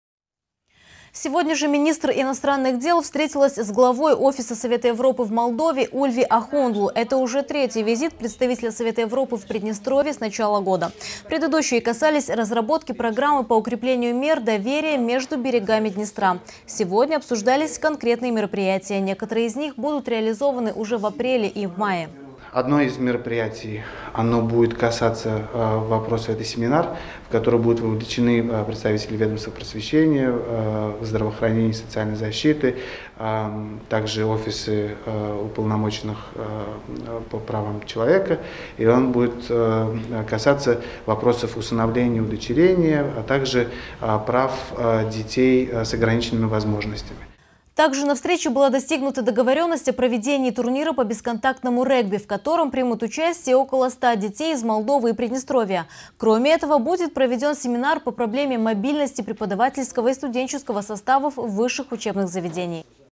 Приднестровье посетил Ульви Ахундлу .flv
Министр иностранных дел Нина Штански встретилась с главой офиса Совета Европы в Молдове Ульви Ахундлу. Это уже третий визит представителя Совета Европы в Приднестровье с начала года.